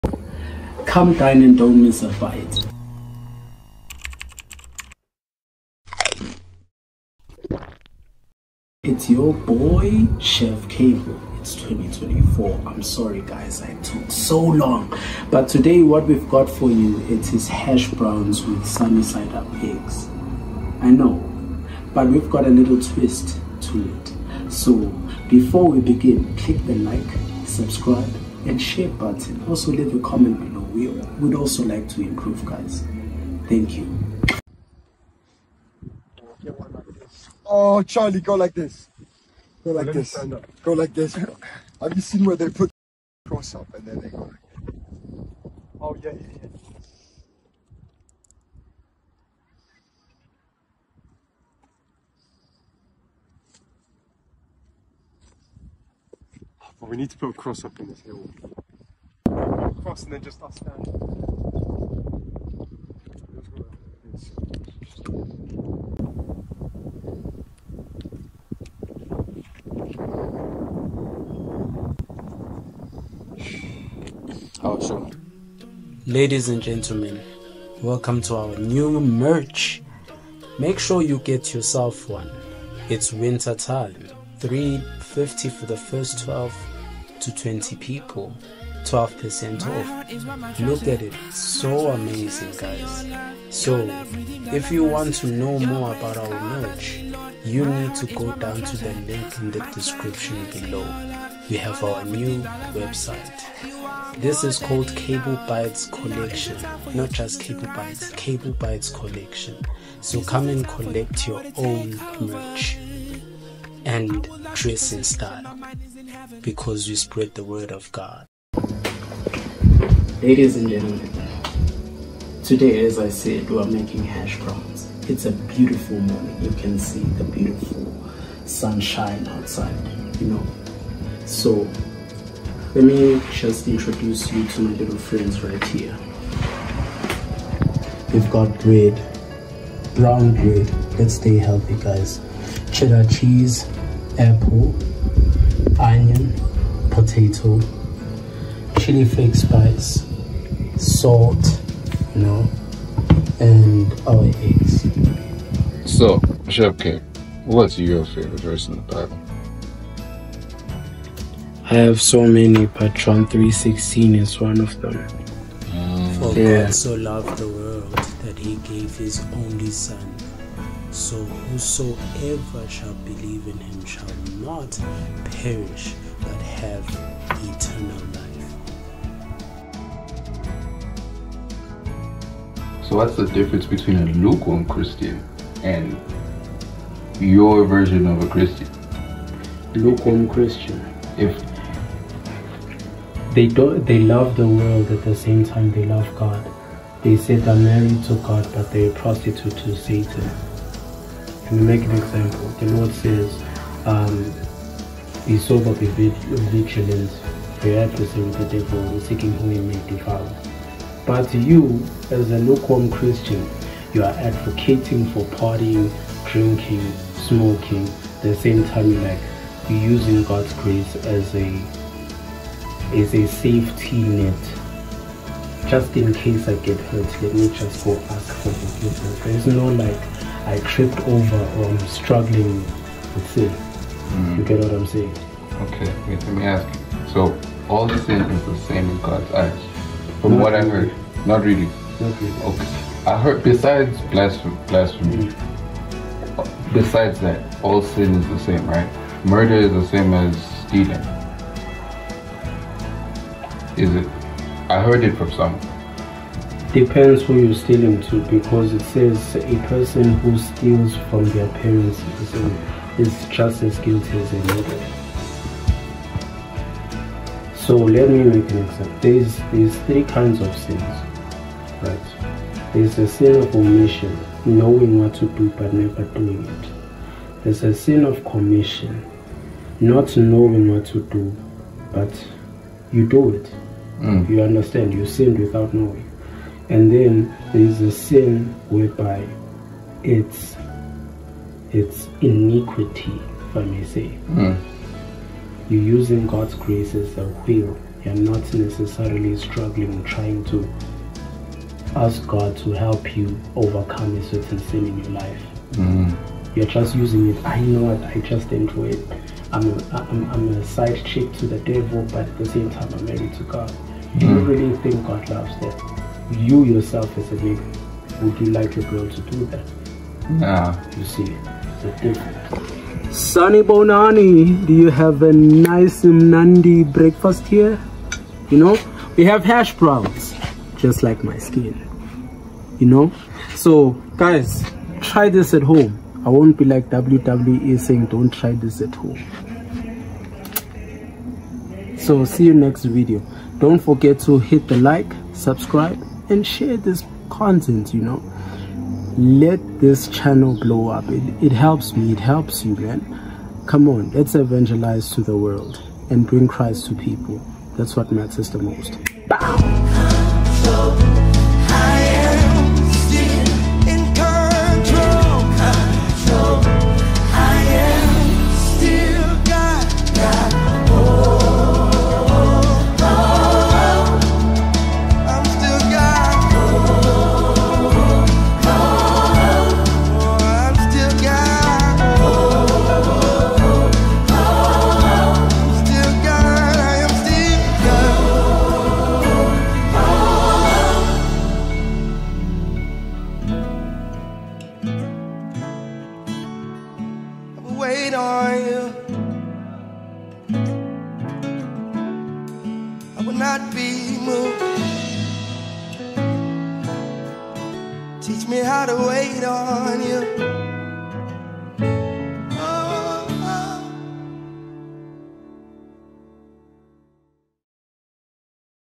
God's Purpose For Life | Best Sunnyside Up Hash Browns You Will Ever Have!
Easy and mouth-watering hash brown recipe.

Ingredients
1 Egg
30 ml oil
1 Onion
2 Potato
1 Apple
30 g Cheddar Cheese
Seasoning

Method
1. Grate your Onion, and Potato and mix them
2. Dice up your apple and cheese.
3. Make a well in your onion and potato then add your apples and cheese and use the seasoning you prefer for taste.
4. Put your hash brown in a hot pan and then add your egg in the well for 2 minutes. Then for another 5 minutes put it in the oven hot from the top.

Time Codes:
00:00 - 0:33 - Intro
00:34 - 1:57 - Clothing Advert
1:58 - 2:35 - Ingredients
2:36 - 5:08 - Cooking Process
5:09 - 5:21 - Dish Presentation
5:22 - 5:59 - Outro
6:00 - 6:59 - Video Credits